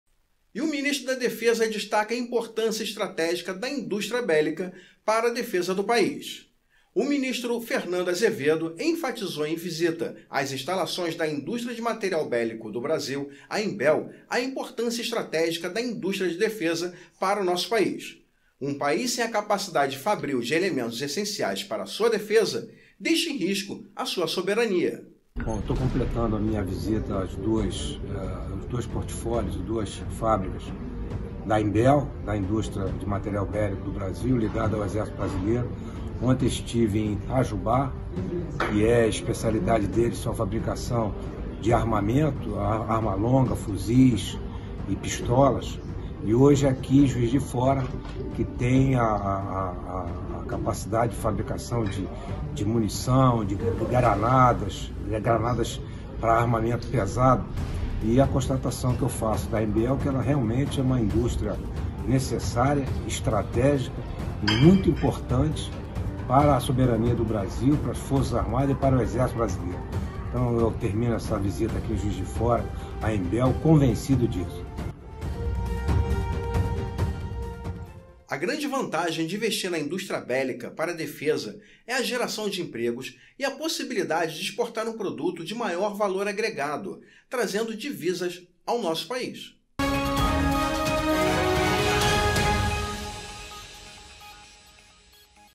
Ministro da Defesa destaca importância da indústria bélica
O ministro da defesa, fernando azevedo, enfatizou em visita às instalações da Indústria de Material Bélico do Brasil (IMBEL), a importância estratégica da indústria da defesa para o Brasil. Um país sem a capacidade fabril de elementos essenciais para sua defesa, deixa em risco a sua soberania.

Apresentação: Luiz Camões
Créditos: Ministério da Defesa

Muito obrigado.